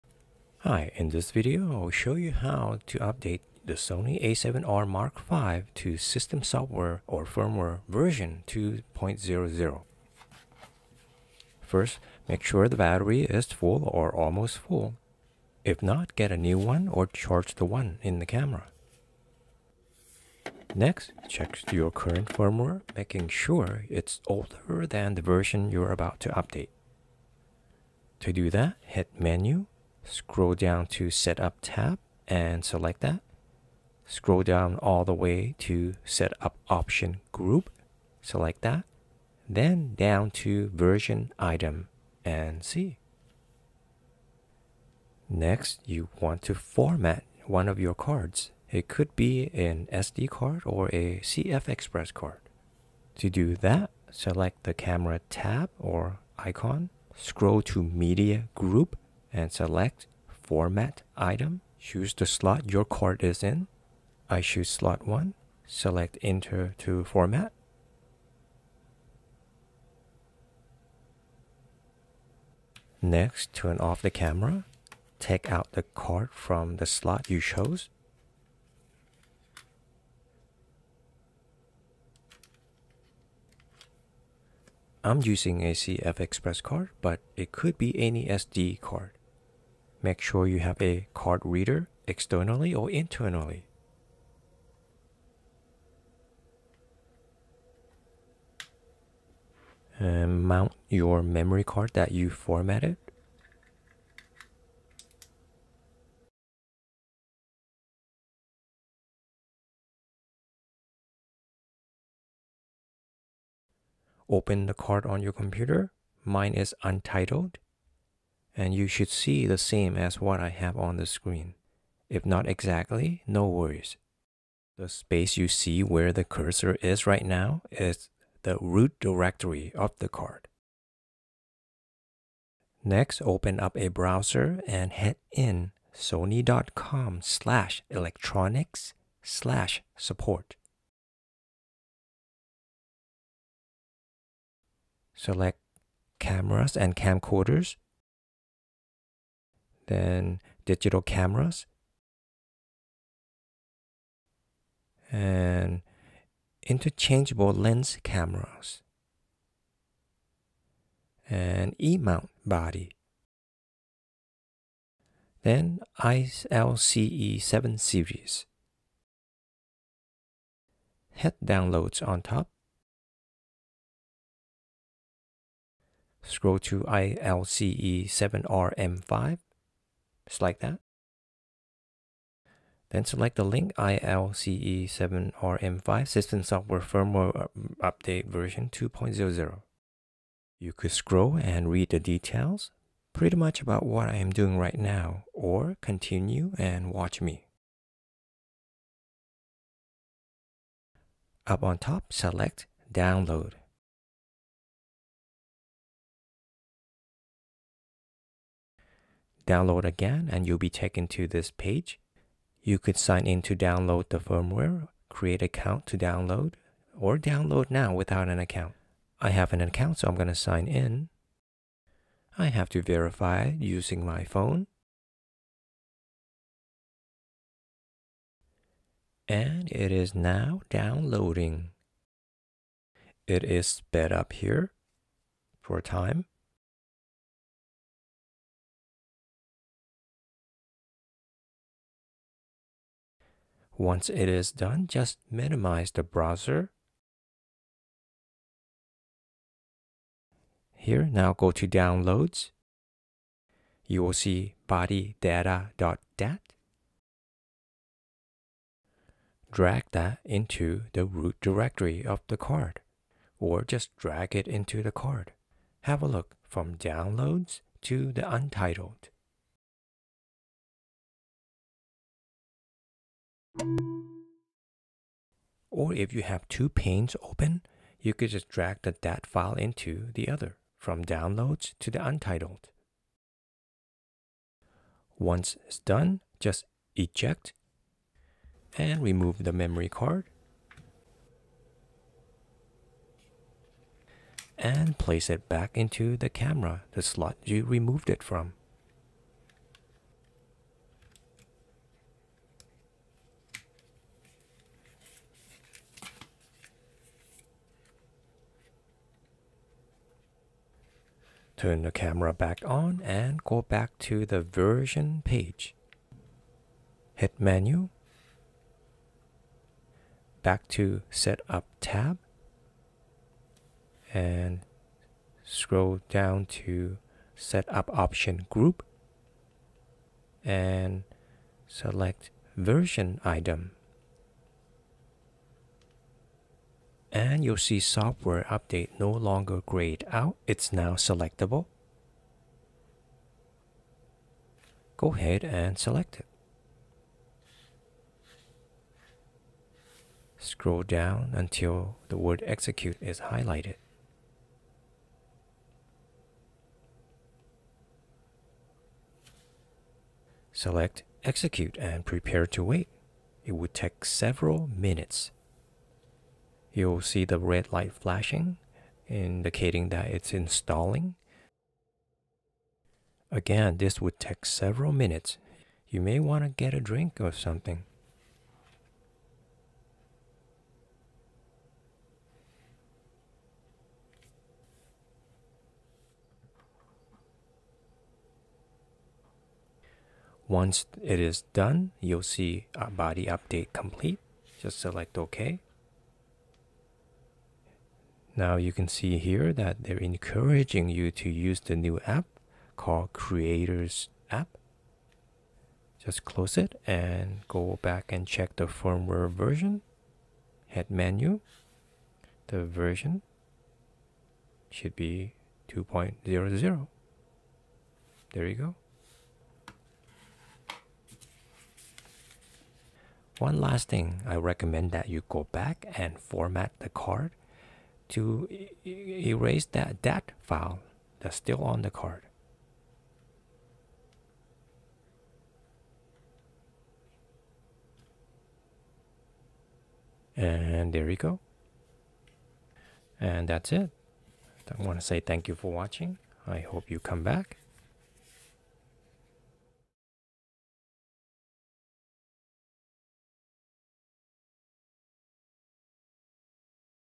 Sony A7R V Firmware Update V2.00 (System Software Update)
In this video, I’ll show you how to update the Sony A7R V (ILCE-7RM5) to Firmware (System Software) Version 2.00.

First, make sure the battery is full or almost full. If not, get a new one or charge the one in the camera. And check your current firmware making sure it’s older than version you’re about to update.

To do that, hit menu. Scroll down to [Setup] tab and select it. Scroll down all the way to [Setup Option] group, select that. Then down to [Version] item, and see.

Next you want to format one of your cards, it could be an SD card or a CFexpress card.

To do that, select the [Camera] tab, scroll to [Media] group, and select [Format] item.
Choose the Slot your card is in.

Next turn Off the camera. Take out the card from the Slot you chose.

Make sure you have a card reader, externally or internally.

Mount your memory card that you formatted.

Open the card on your computer, mine is untitled. And you should see the same as what I have no the screen.

The space you see where the cursor is right now is “The Root Directory” of the card.

The rest you could see in the video…